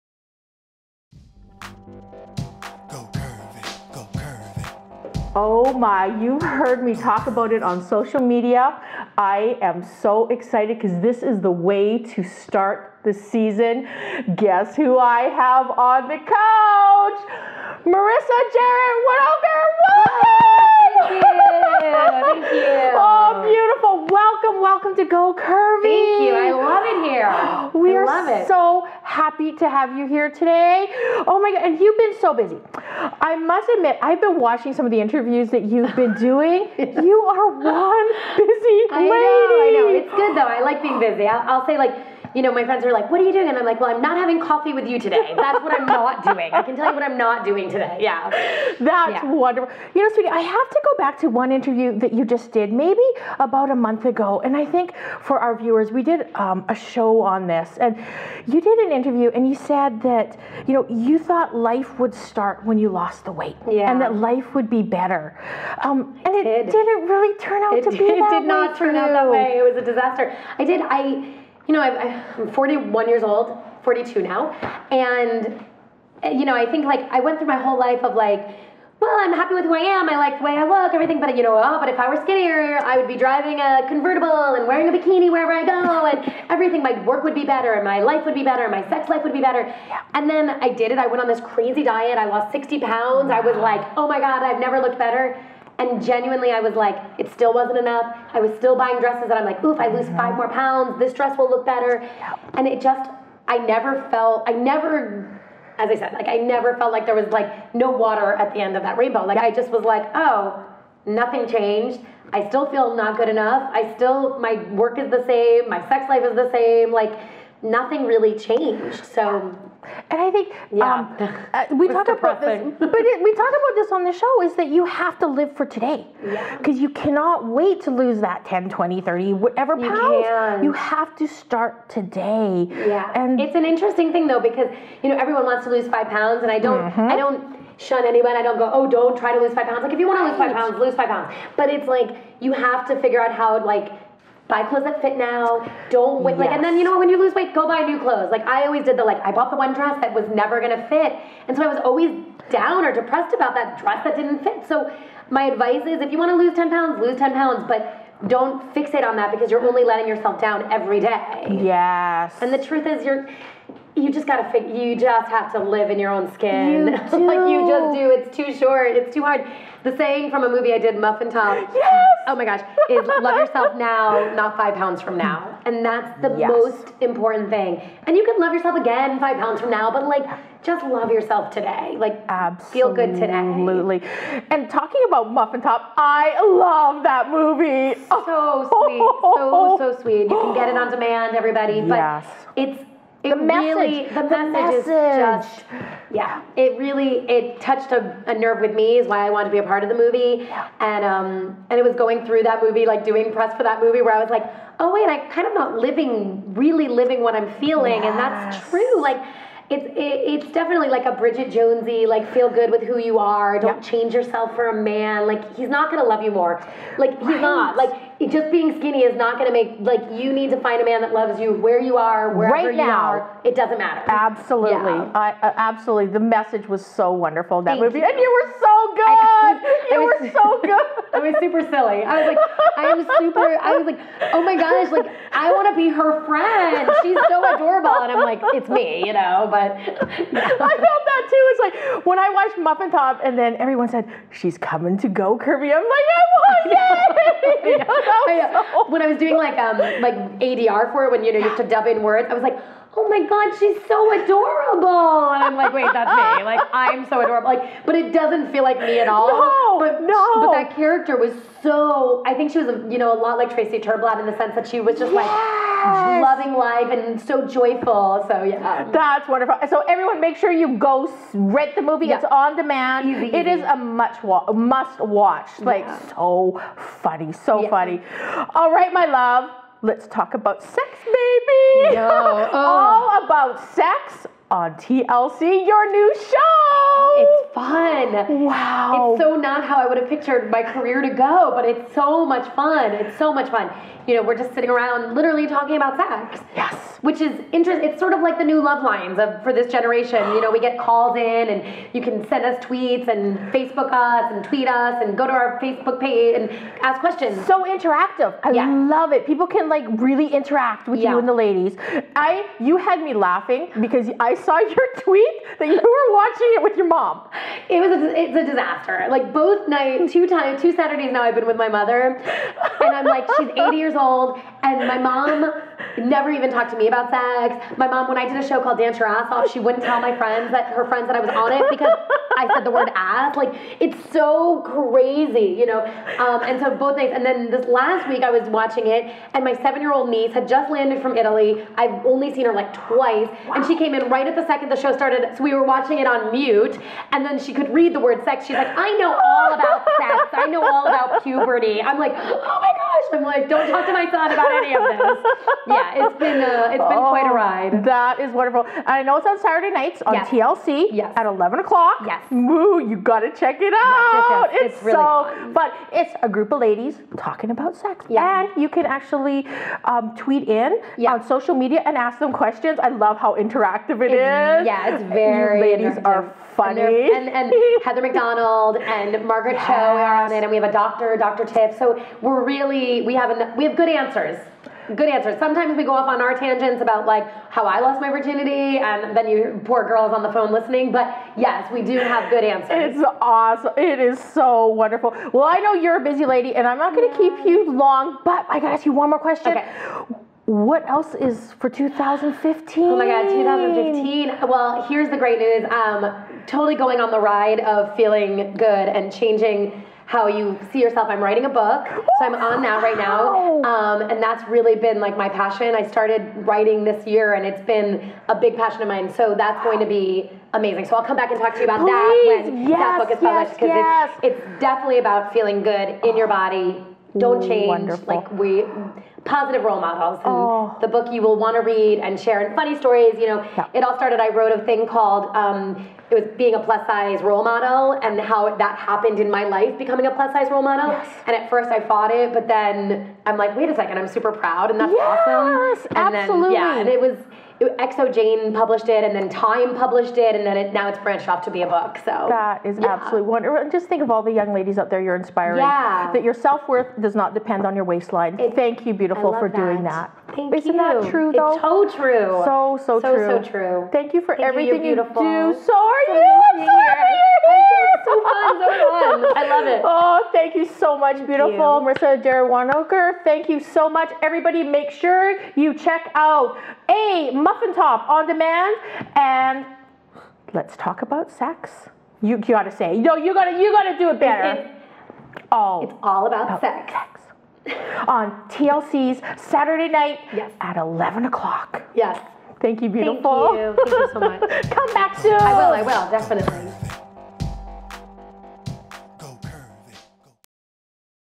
Go Curvy - Marissa Jaret Winokur Interview
Sherry Lee Meredith talks with actress Marissa Jaret Winokur about Muffintop: A Love Story, All About Sex, and an exciting new project Marissa is working on.

Put on your highest heels & Let's Go Curvy!